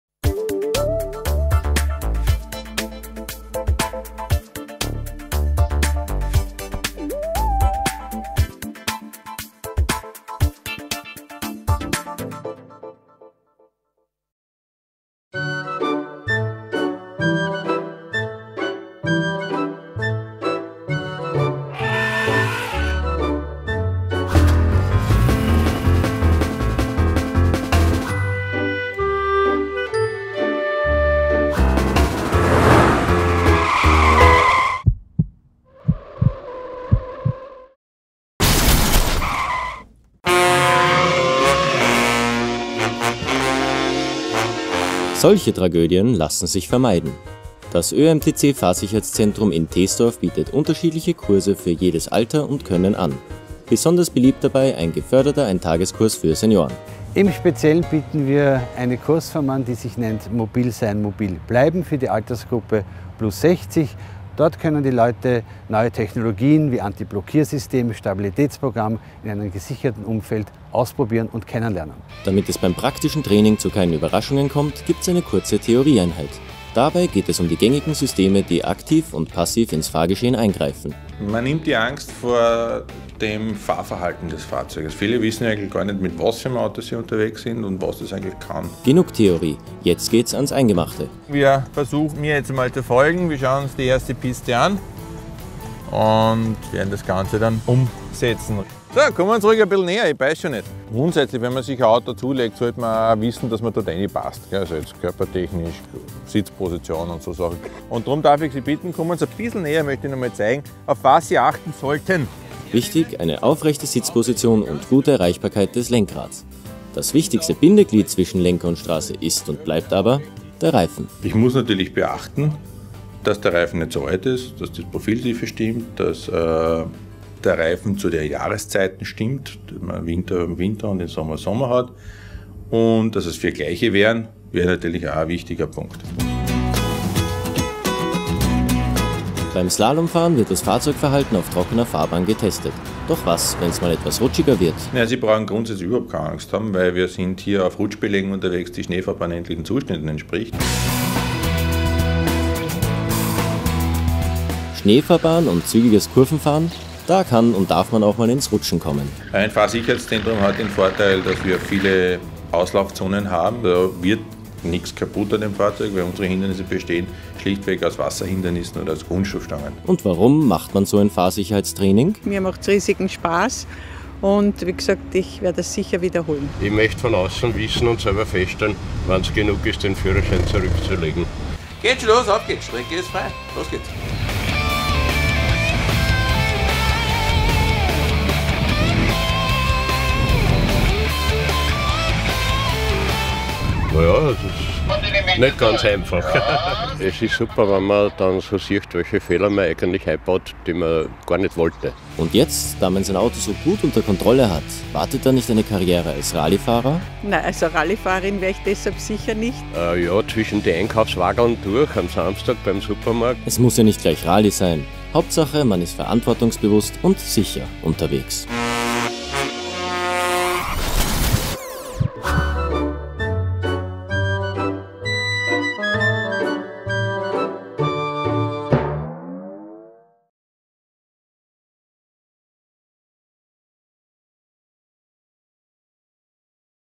Fahrsicherheitstraining für SeniorInnen
Action auf vier Rädern: Im ÖAMTC-Fahrsicherheitszentrum in Teesdorf werden unterschiedliche Kurse für jedes Alter und Können angeboten. Besonders beliebt: der Ein-Tages-Kurs für SeniorInnen.